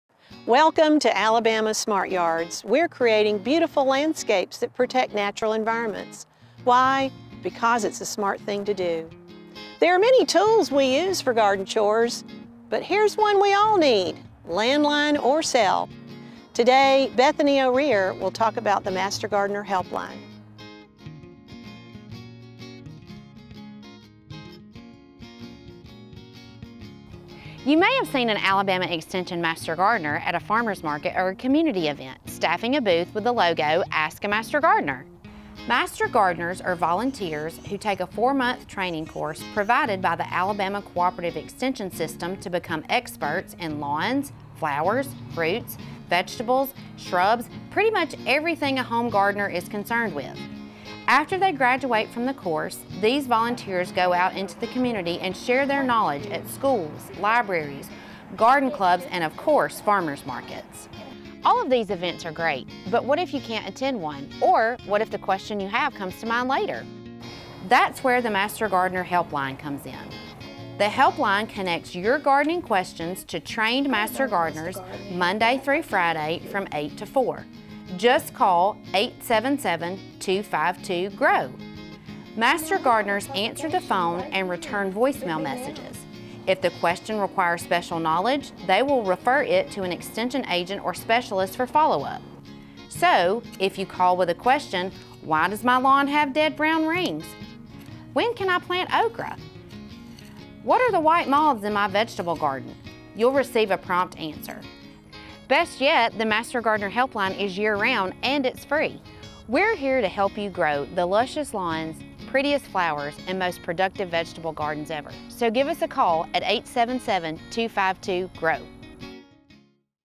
Alabama Smart Yards: Master Gardener Helpline
Alabama Smart Yards wants to let you know about a great tool for your garden- the Master Gardener Helpline!